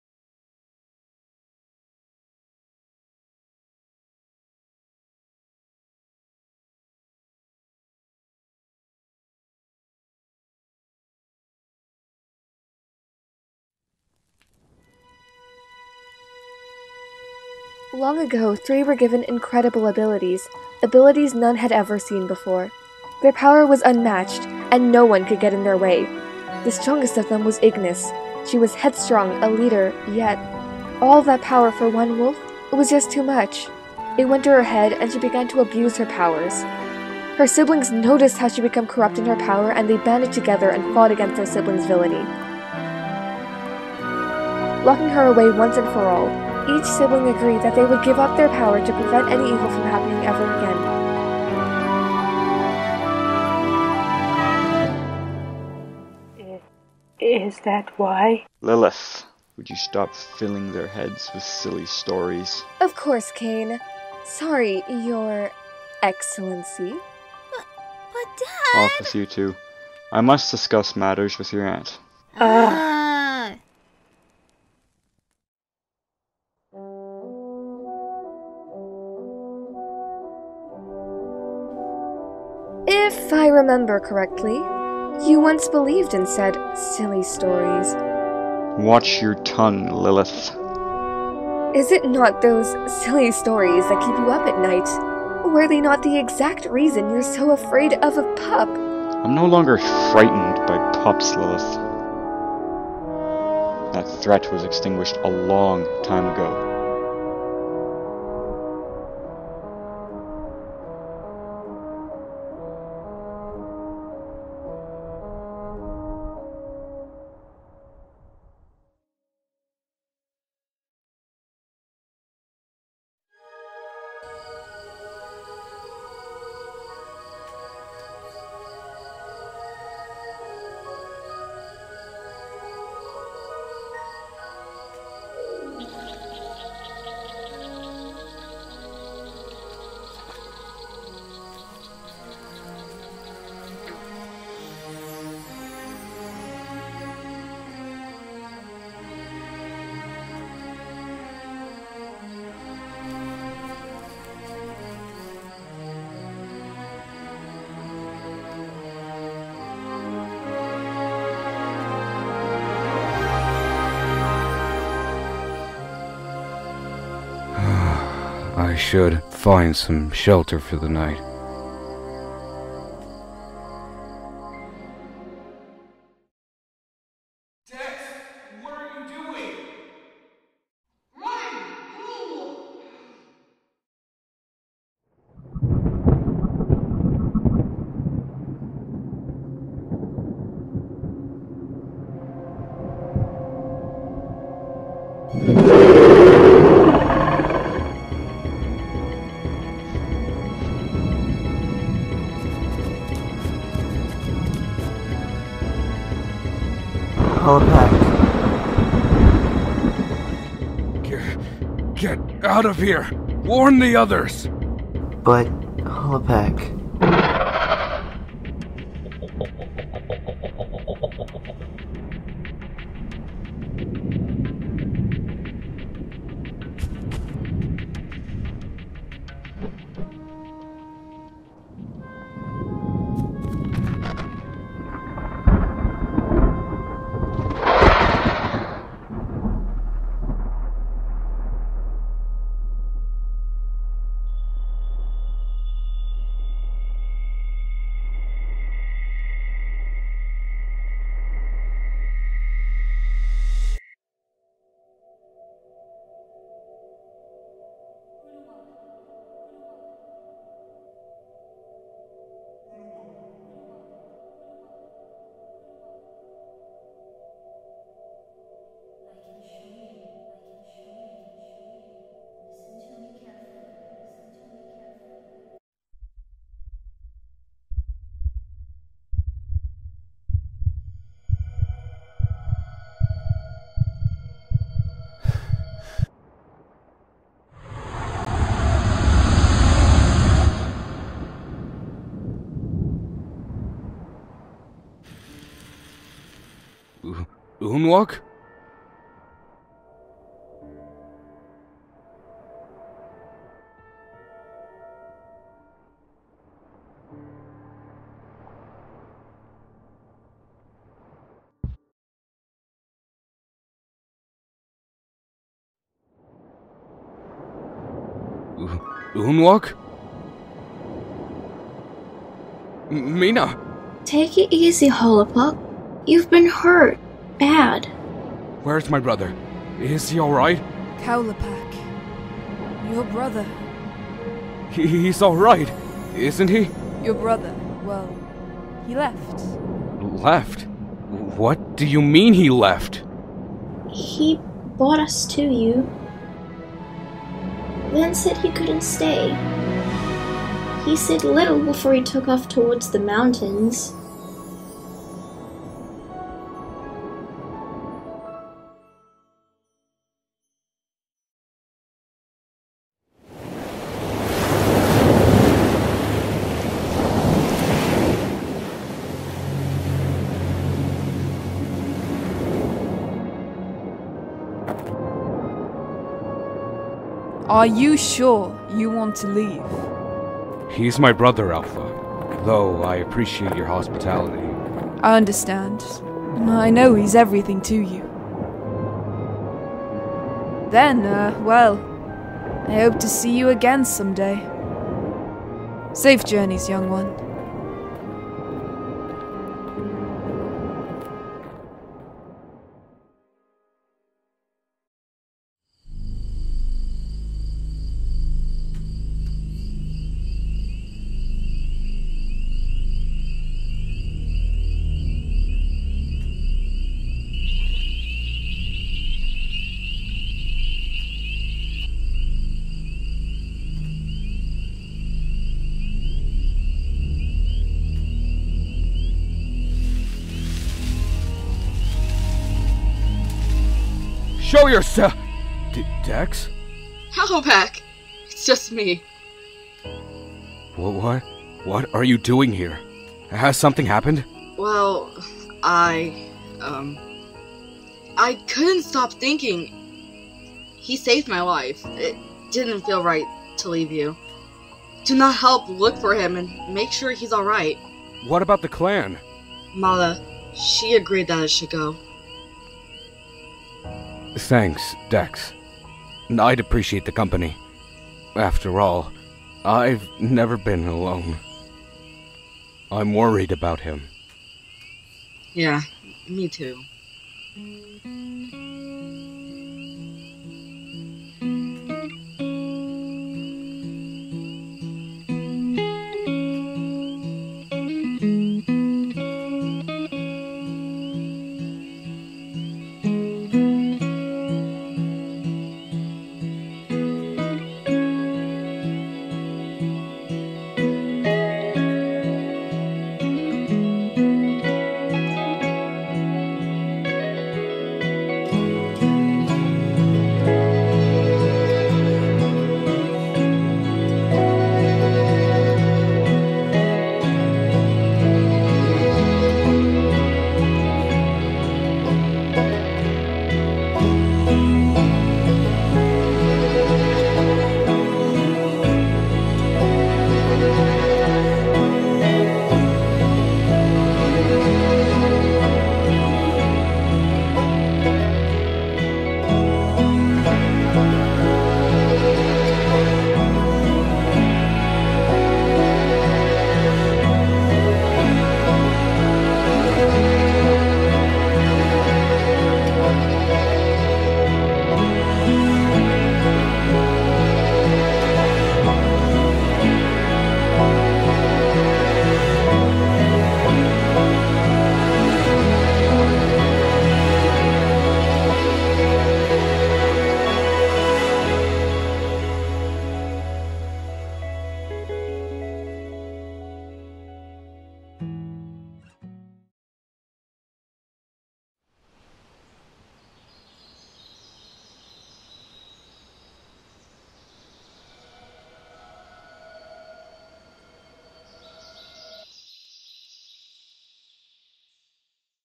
Fratres Episode 3 || Dissilunt
|| Warning ||

-Mild cartoon violence
-Flashing images

After 8 Months. Episode 3 is here!
I can't express how incredibly proud of my team I am. You all did an amazing job!
This was by far one of my favourite and most difficult episodes to animate.

Keep an eye out for more updates on the series 👀

Episodes

NEXT: Coming Soon

Fizzle as Unnuak

Art Team

Fratres Crew